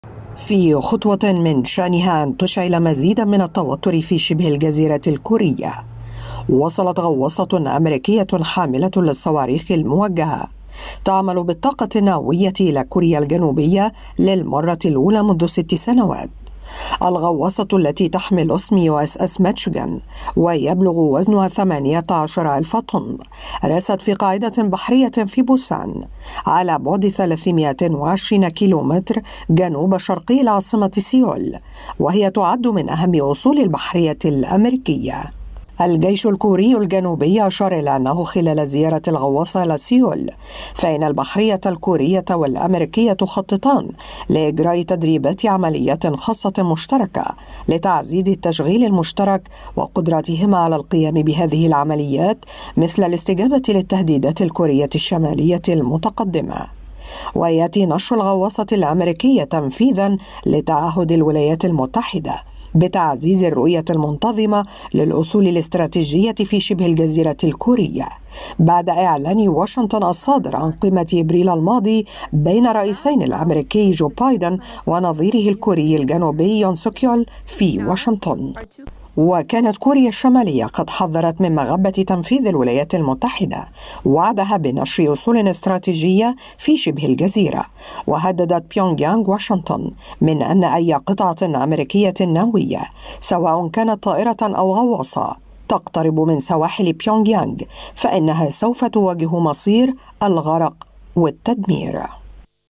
وصول غواصة أمريكية حاملة للصواريخ تعمل بالطاقة النووية إلى كوريا الجنوبية
غرفة الآخبار| وصول غواصة أمريكية حاملة للصواريخ تعمل بالطاقة النووية إلى كوريا الجنوبية